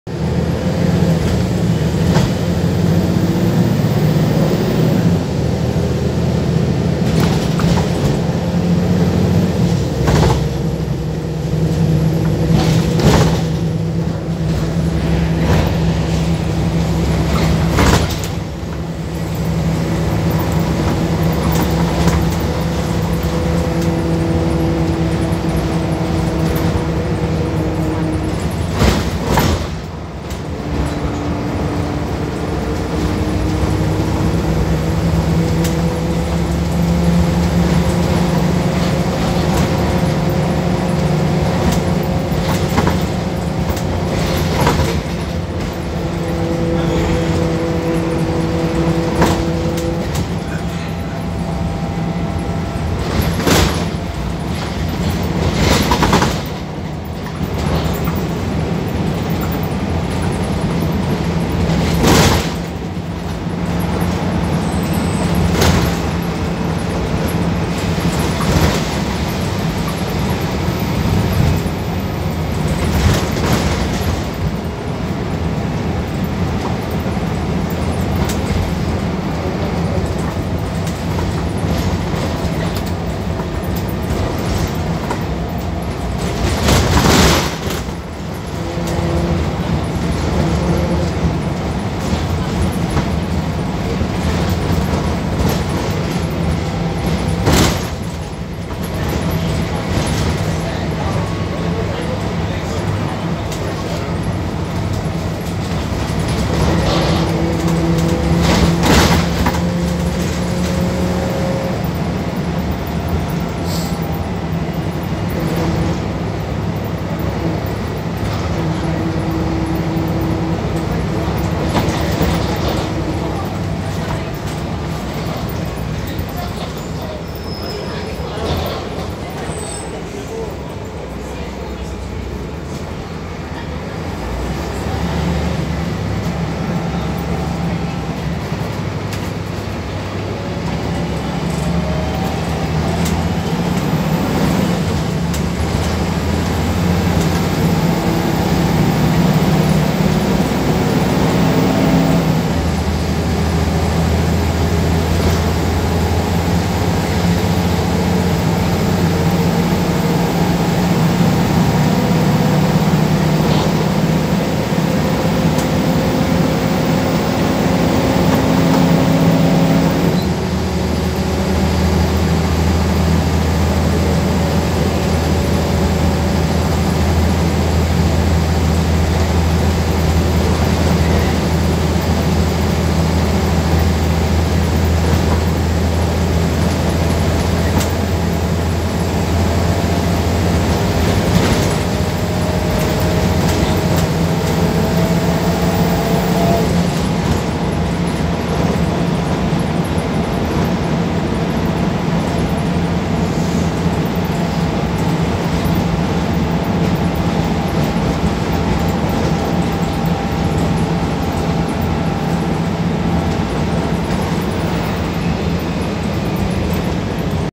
First Bath 66885 MX05 CLF
Here we see a journey on B7RLE 66885 MX05 CLF seen on route D2 to Bath.